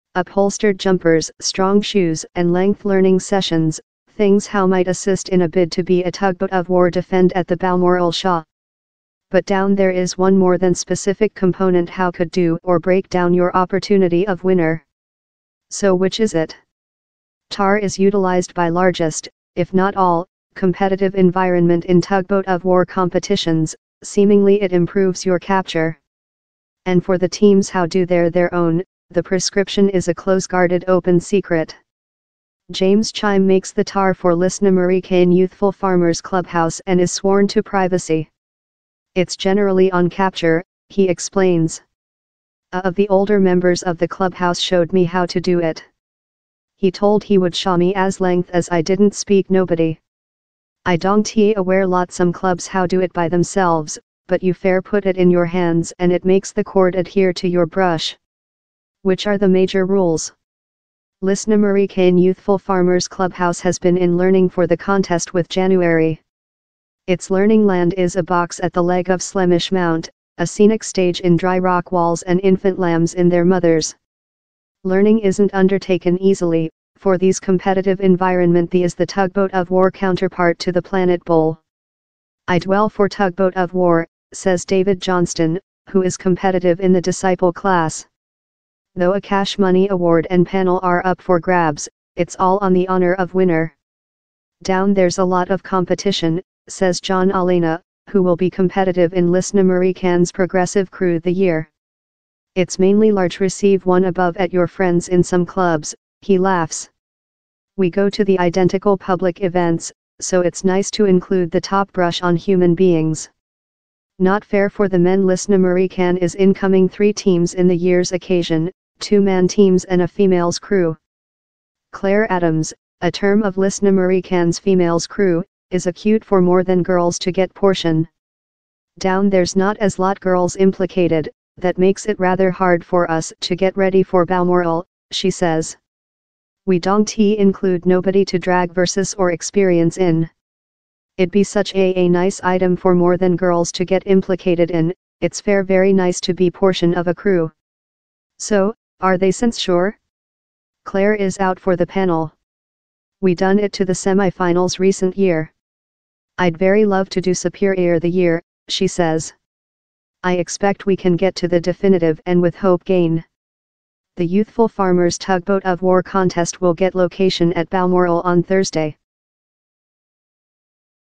Balmoral Shaw: The Fight To Be Tugboat-of-war Champions
From secret recipes to special boots, training for the tug-of-war at Balmoral is a serious business.